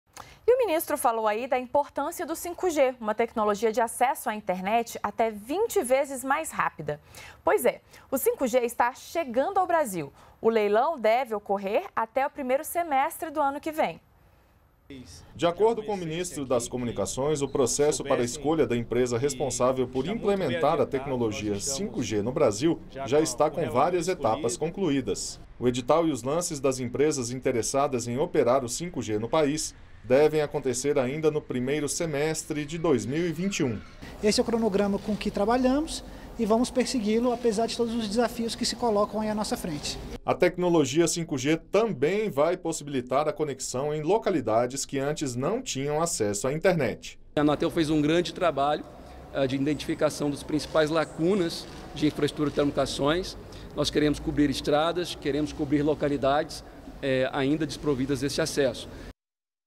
Leilão do 5G será no primeiro semestre de 2021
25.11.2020 - A tecnologia 5G está chegando ao Brasil. O leilão deve ocorrer até o primeiro semestre do ano que vem.